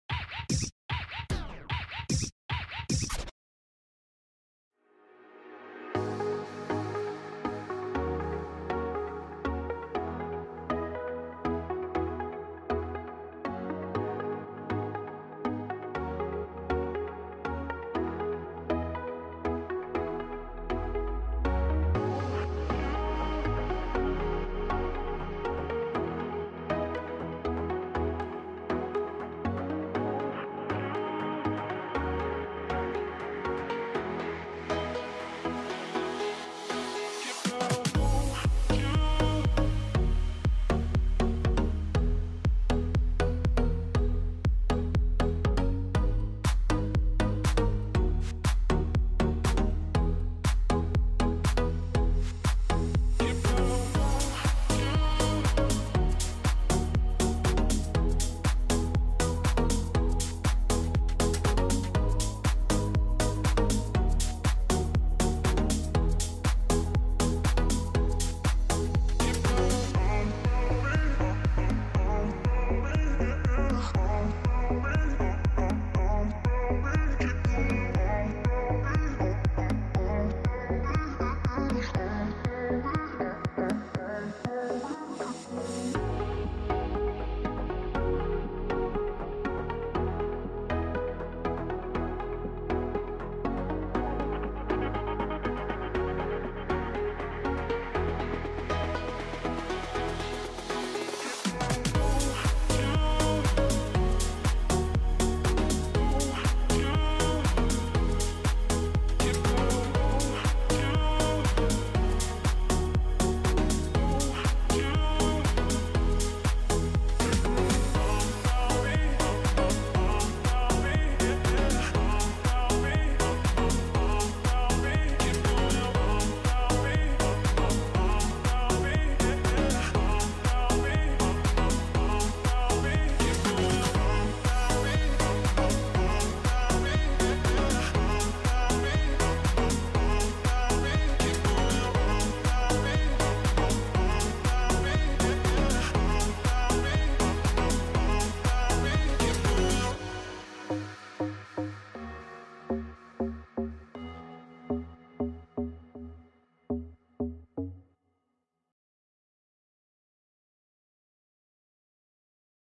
【Snow White】Showing her navel
Snow White with her navel was charming.